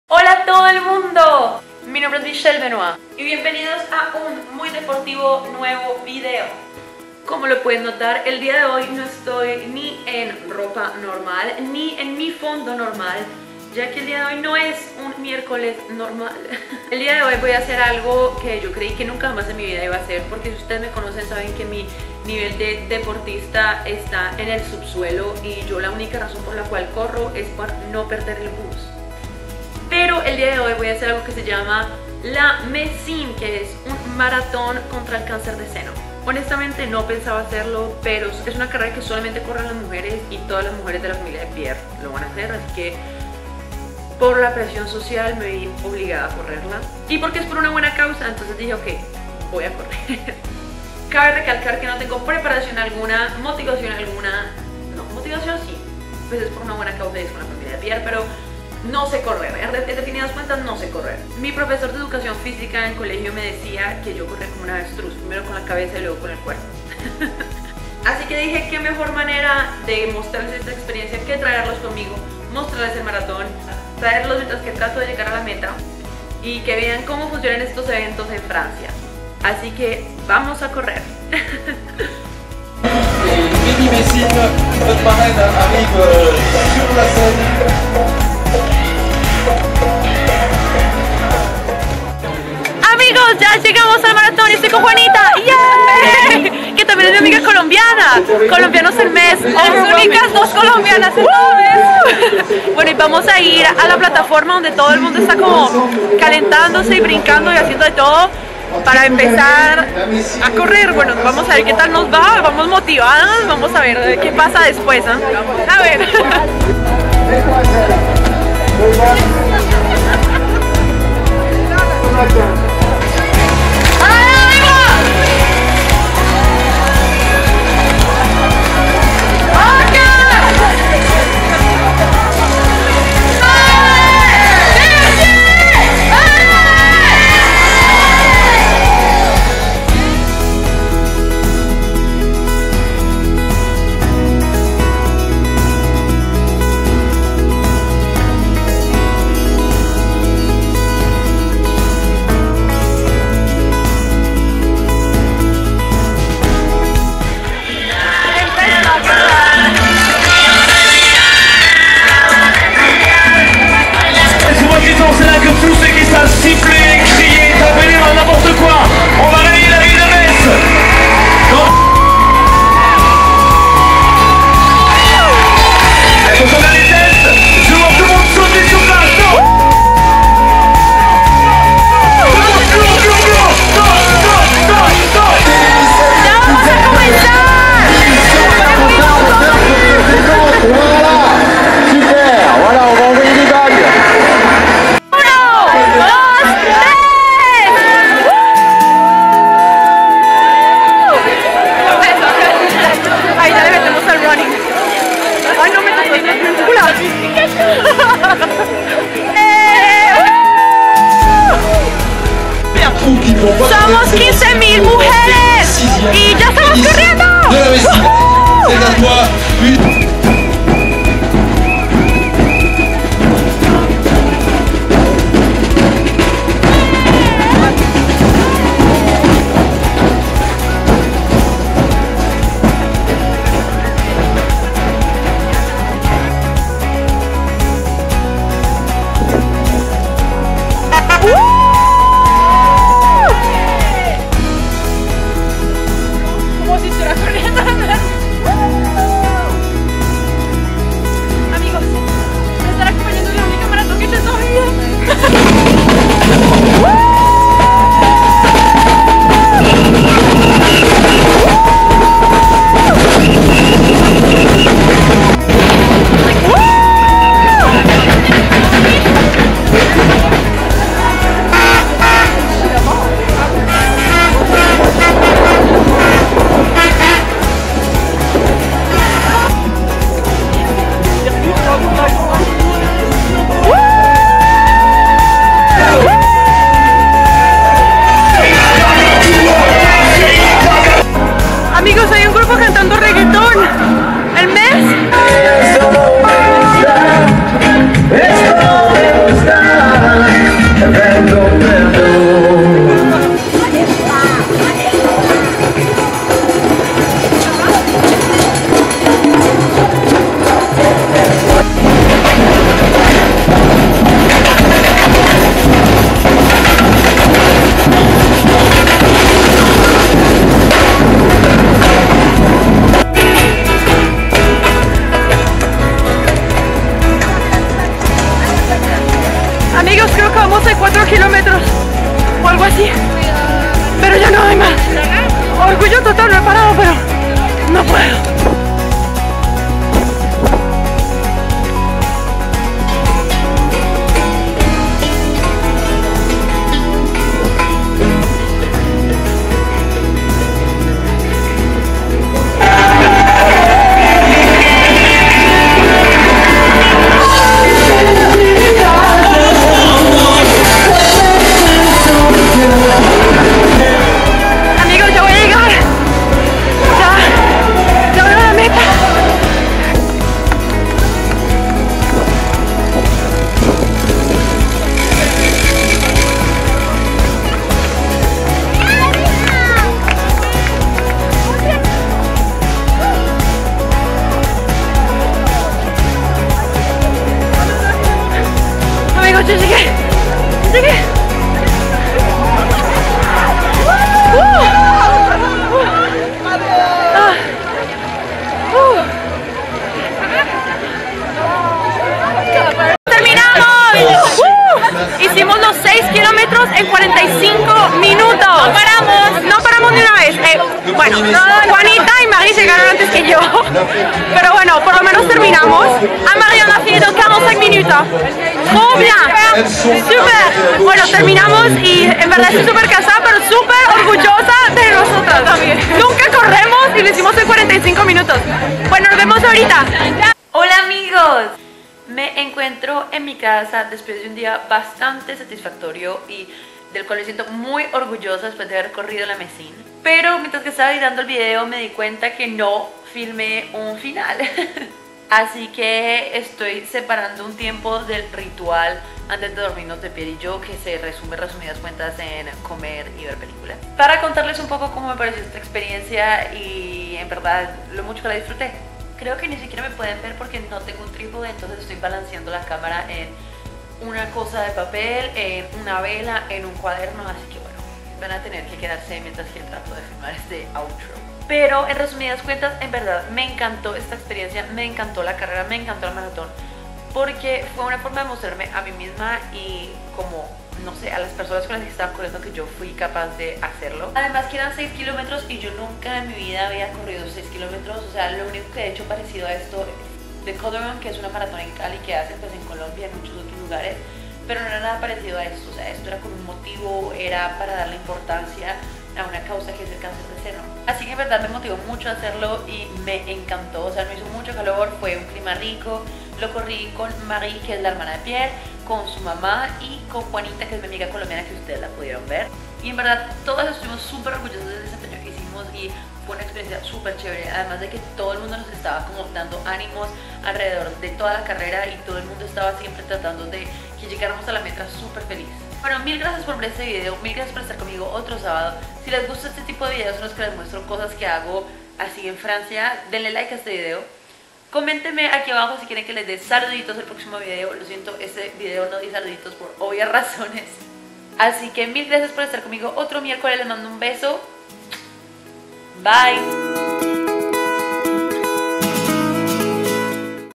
¡MARATÓN CONTRA EL CANCER DE SENO! La Messine 2017 // Michelle Benoit
Hola, salut, hello!!
COSITAS INTERESANTES AQUI ABAJO!!
Hola amigos!!
El día de hoy hice algo que nunca pensé hacer! corrí 6 kilómetros en Francia para la prevención del cáncer de seno! fue una experiencia super bonita y enriquecedora, ademas de que nos probamos a nosotras mismas que podemos ir mas allá de nuestros propios limites! si les gustó este video denle like y compartan! los amo
picos pues!